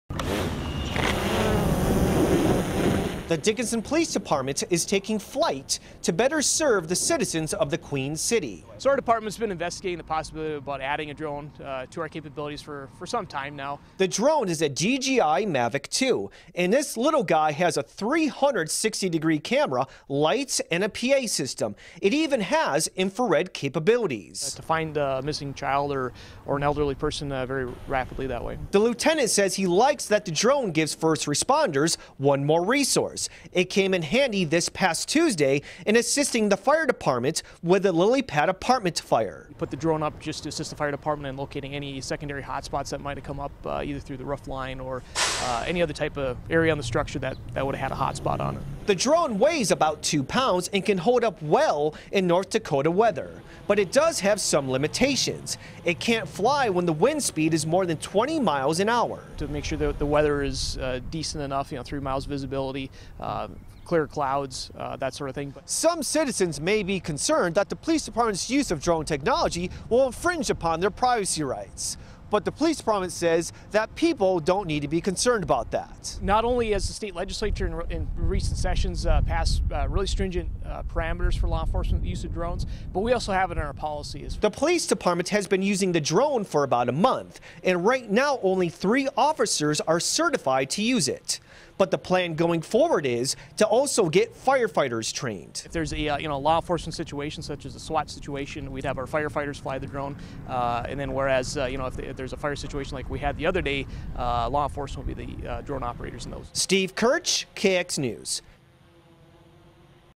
Police department takes public safety to new heights in Dickinson
Dickinson, ND (KX News) The Dickinson Police Department is taking public safety to new heights to better serve the citizens of the Queen City.

“So our department has been investigating the possibility of adding a drone to our capabilities for some time now,“ said Lt. Mike Hanel, Dickinson Police Department.